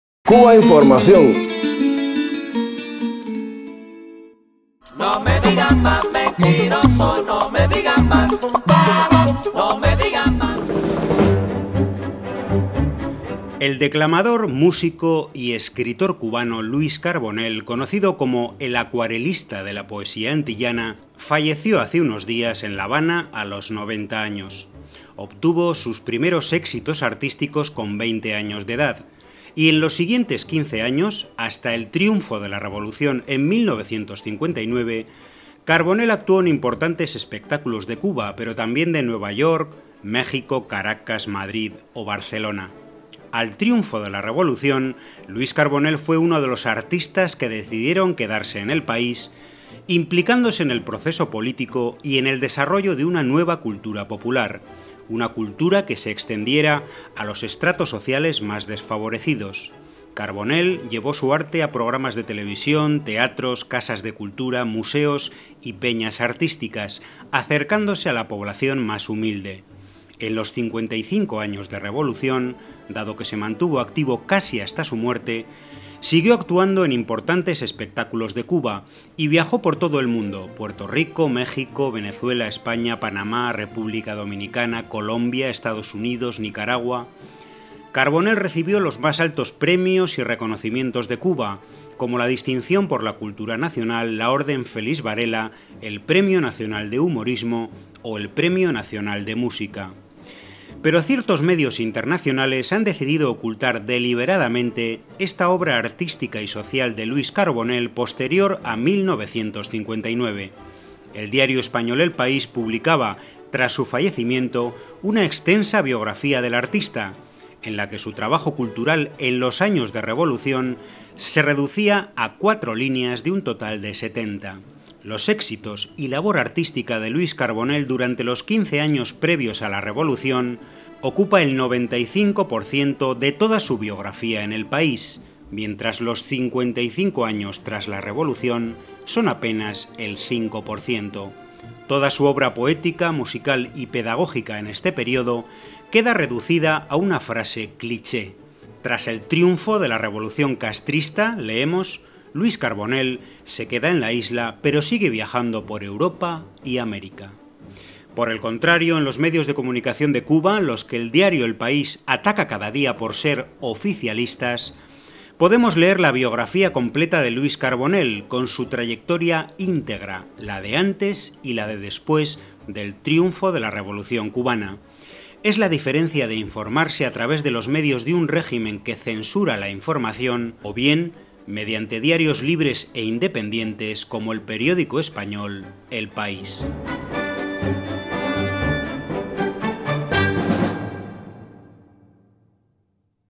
Luis Carbonell en El País: desaparecen 55 años de Revolución de su biografía
Ciertos medios internacionales han decidido ocultar deliberadamente la obra artística y social del declamador y poeta cubano Luis Carbonell posterior a 1959. El diario español El País publicaba, tras su fallecimiento, una extensa biografía del artista, en la que su trabajo cultural en los años de Revolución se reducía a 4 líneas de un total de 70. Los éxitos y labor artística de Luis Carbonell durante los 15 años previos a la Revolución ocupan el 95 % de toda su biografía en El País, mientras los 55 años tras la Revolución son apenas el 5 %. Toda su obra poética, musical y pedagógica en este período, queda reducida a una frase cliché: "Tras el triunfo de la revolución castrista (...) --leemos-, (Luis Carbonell) se queda en la isla, pero sigue viajando por Europa y América". Edición: Esther Jávega.